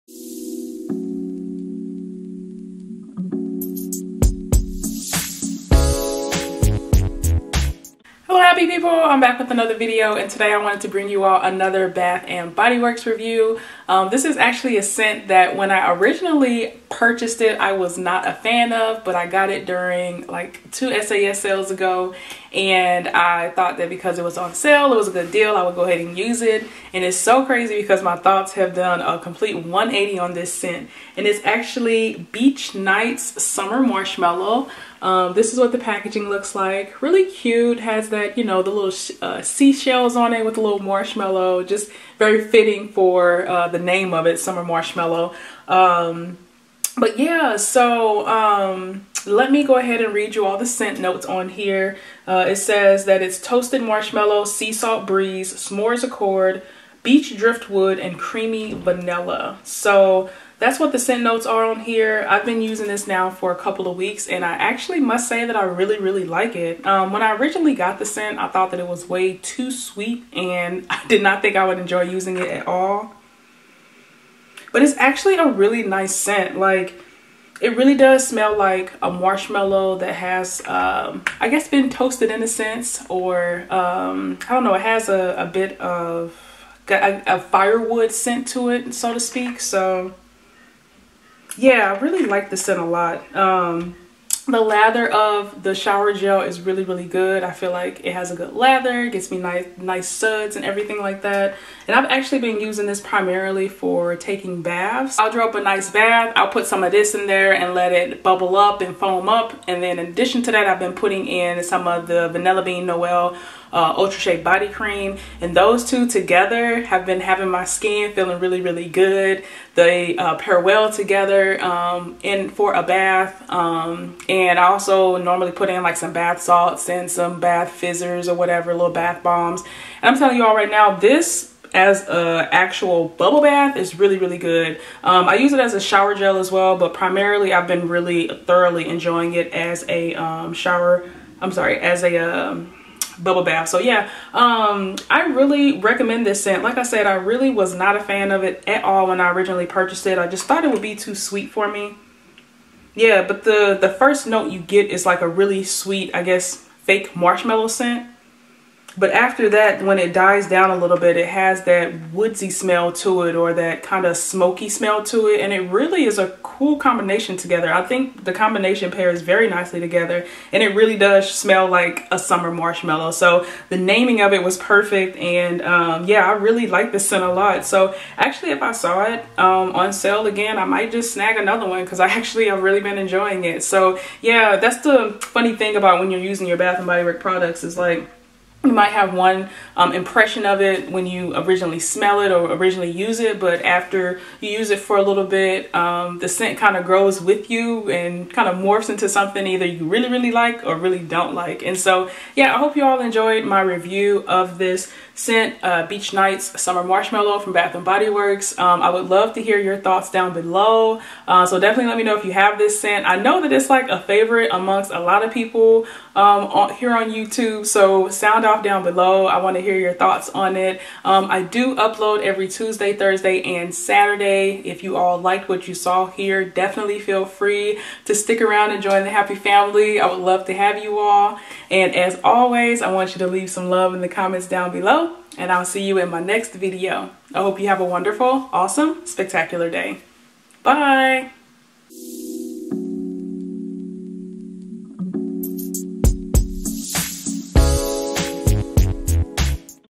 Bath and Body Works Beach Nights Summer Marshmallow | BBW
Hello happy people, in this video I give my honest thoughts on the Bath and Body Works scent Beach Nights Summer Marshmallow.  Please leave your thoughts on this scent below. Enjoy.

Beach Nights Summer Marshmallow Shower Gel

Beach Nights Summer Marshmallow Lotion

Beach Nights Summer Marshmallow Body Cream